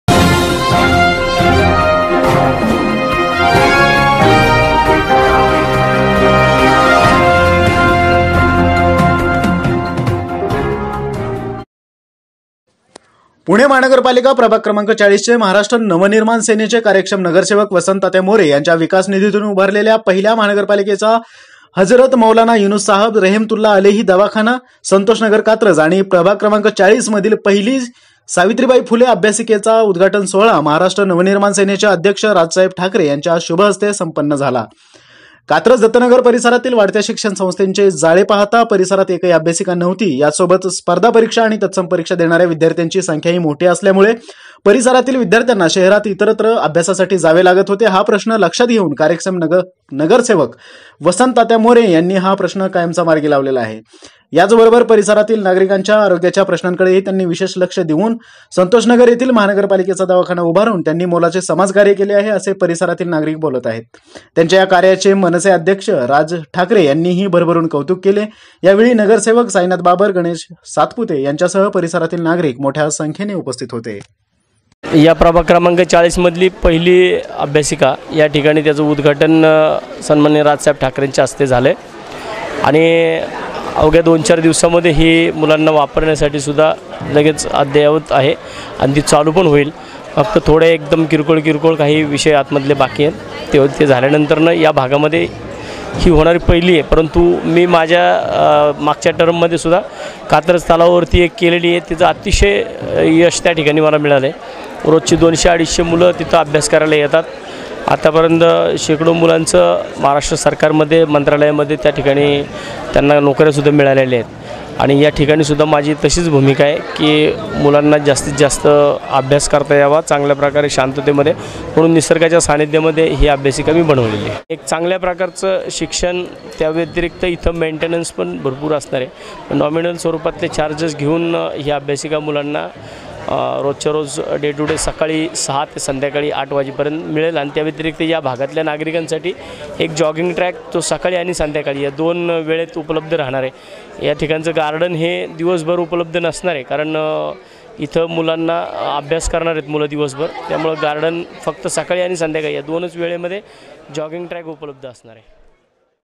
मनसे अध्यक्ष मा.राज ठाकरे यांच्या शुभ हस्ते,दत्तनगर/कात्रज येथे अभ्यासिका व दवाखान्याचे उद्घाटन
राज ठाकरे यांच्या हस्ते दवाखाना आणि अभ्यसिकेचे उदघाटन.!

युवा संवाद न्यूज पुणे (प्रतिनिधी):
          पुणे महानगरपालिका प्रभाग क्रमांक ४०चे  महाराष्ट्र नव-निर्माण सेनेचे नगरसेवक वसंत मोरे यांच्या विकास निधीतून उभारलेल्या पहिला महानगरपालिकेचा हजरत मौलाना यूनुस साहब रहमतुल्ला आलेही दवाखाना संतोषनगर कात्रज आणि प्रभाग क्रमांक ४० मधील पहिली सावित्रीबाई फुले अभ्यासिकेचा उद्घाटन सोहळा महाराष्ट्र नवनिर्माण सेनेचे अध्यक्ष राज ठाकरे यांच्या शुभहस्ते संपन्न झाला.
       कात्रज-दत्तनगर परिसरातील वाढत्या शिक्षण संस्थांचे जाळे पाहता परिसरात एकही अभ्यासिका नाही. याचसोबत स्पर्धा परीक्षा आणि तत्सम परीक्षा देणाऱ्या विद्यार्थ्यांची संख्याही खूप मोठी आहे. त्यामुळे परिसरातील विद्यार्थ्यांना शहरात इतरत्र जावे लागत होते. हा प्रश्न लक्षात घेऊन कार्यक्षम नगरसेवक वसंत मोरे यांनी हा प्रश्न कायमचा मार्गी लावलेला आहे. त्याचबरोबर परिसरातील नागरिकांच्या आरोग्याच्या प्रश्नाकडे ही त्यांनी विशेष लक्ष दिल्याचे दिसते आहे. संतोष नगर येथे महानगरपालिकेचा दवाखाना उभारून त्यांनी मोलाचे समाजकार्य केले आहे. असे परिसरातील नागरिक भावना व्यक्त करत आहेत. त्यांच्या या कार्याचे मनसे अध्यक्ष राज ठाकरे यांनीही भरभरून कौतुक केले.
        यावेळी नगरसेवक साईनाथ बाबर, गणेश सातपुते यांच्यासह परिसरातील नागरिक मोठ्या संख्येने उपस्थित होते.!

दक्षिण पुणेराज ठाकरेउदघाटन.
युवा संवाद न्यूज#गतिमान युगाकडे.
बातम्या आणि जाहिरातींसाठी संपर्क-९६२३४१८४१९.